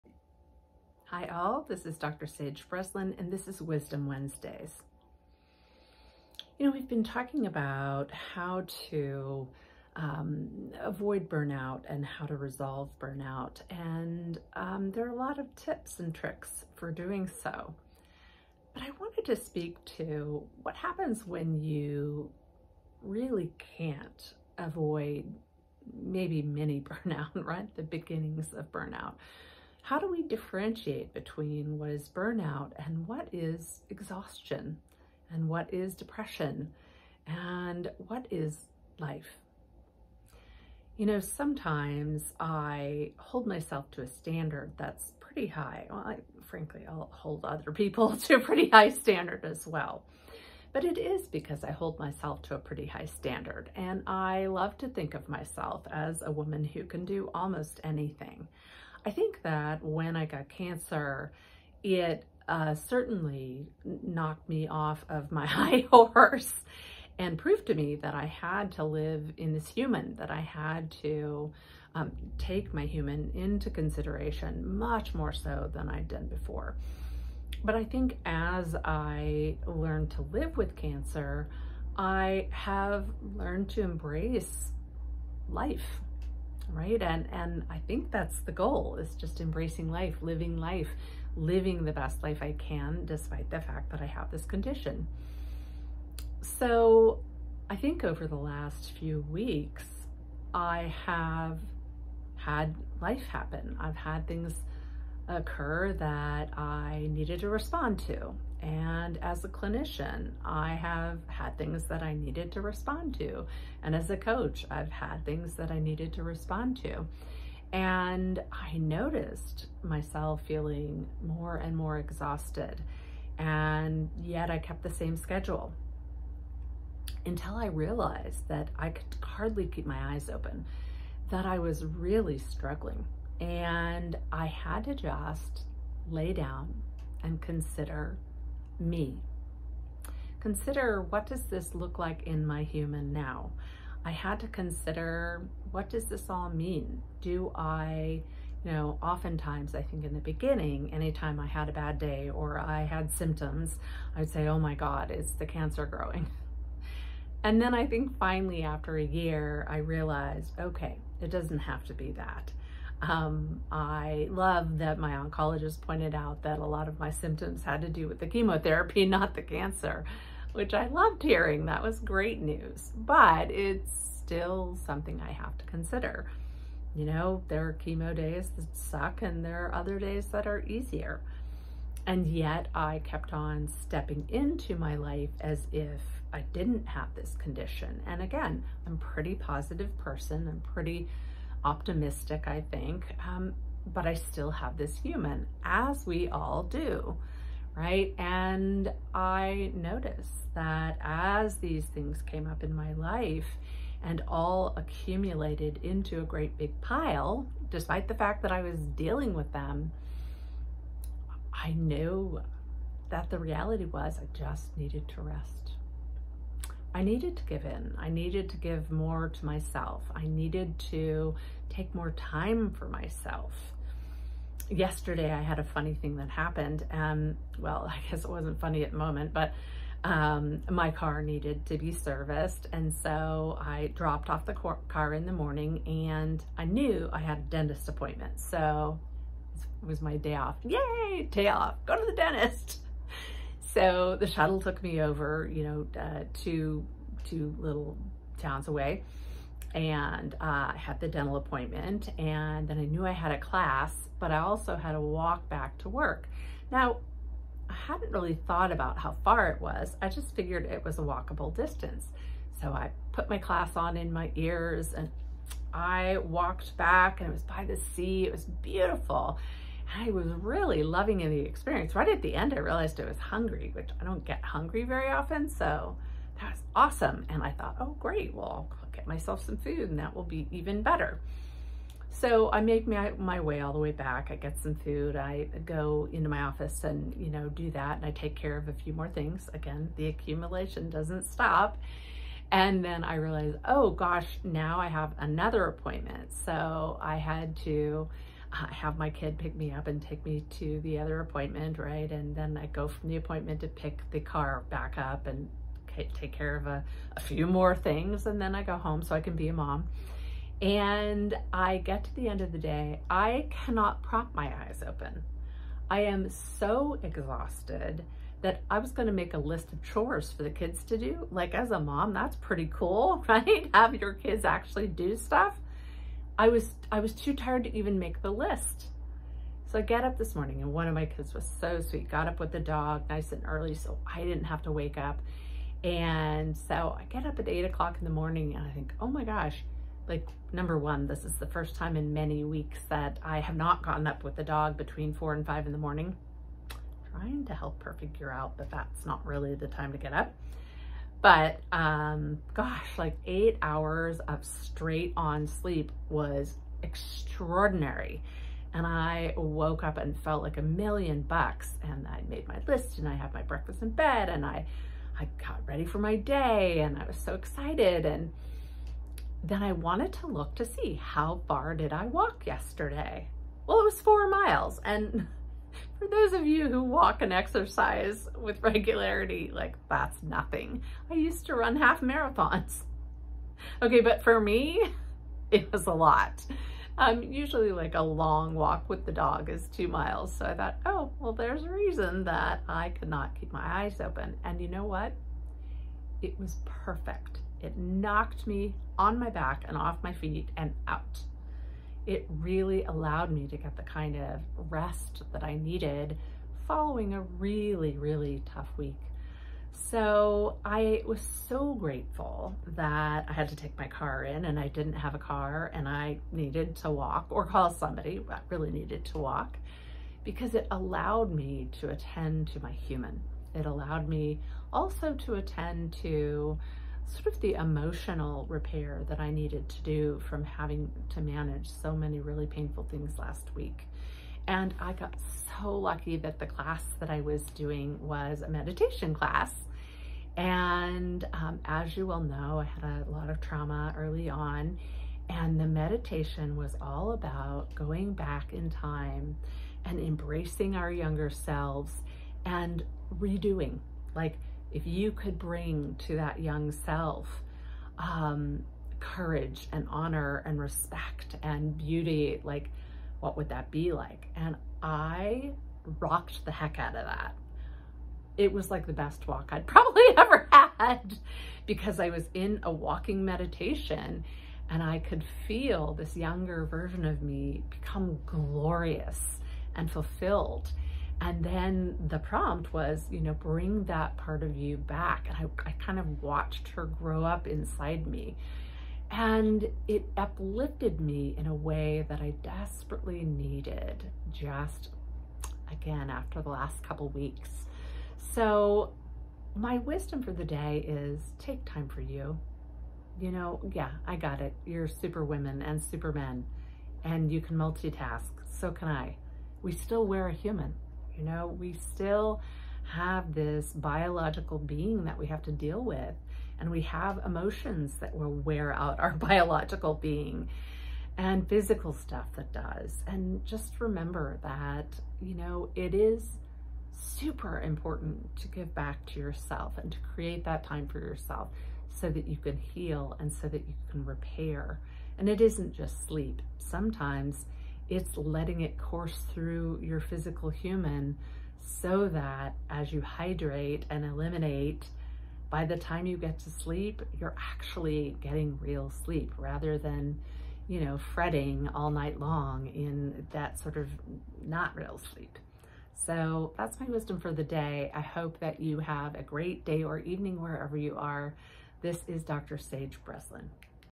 Wisdom Wednesdays: Burnout, Exhaustion, Depression or LIFE?
Learning to discern between exhaustion, burnout and depression is important, but what about when LIFE just happens.

I hold myself to a pretty high standard- honestly, I hold others to the same high standard- but sometimes I have to remember that there are times when I need to lower the bar. I need to remember that I have a HUMAN to consider- even when I feel like I can do almost anything!

Having cancer helped me do a better job keeping my human needs in mind, but after about a year, I learned to LIVE with cancer. I've spent less time in fear of dying, and more time embracing LIFE. But, there are times when I forget that it's a big toll on my body. And when LIFE HAPPENS, I have to make time for recovery!

It's been a rough two weeks of LIFE. I showed up for each of the things I need to do- as a friend, as a mom, as a coach, as a therapist, and as a woman, but DANG, y'all! that accumulation whooped me! I stopped sleeping so well, I didn't feel great, and life just wasn't as fun as it can be.

Yesterday, I got an opportunity- one that could've caused me to be pissy but that I turned into the sweetest lemonade: I had to take my car into the shop- so I let the shuttle take me to the dentist a few towns over, then as I plugged into my meditation class, I walked back to my office. It was glorious walking by the sea and the meditation was designed to help me embrace my teenage self. Many of you know that that self was trafficked, and learned to feel like a possession. So in the course of walking back to my office, I wrapped her in courage and respect and adoration, and as her smile grew, I could feel the transition inside me. By the time I got to my office (to do a few tasks before going to the oncologist where I got 5 vaccines!), I felt a joy I hadn't been able to access for weeks.

And last night, I fell into the deepest 8 hours of sleep I've had in many months.

I awoke today refreshed and eager to get into work- and into my life.

My wisdom for the day? Take the time you need for recovery when LIFE HAPPENS. It doesn't have to be a sign of exhaustion or burnout or depression, sometimes when life happens, you've got to let it all roll through, and then you need to take time to restore your emotional and physical being.

Take time for you- you're worth it!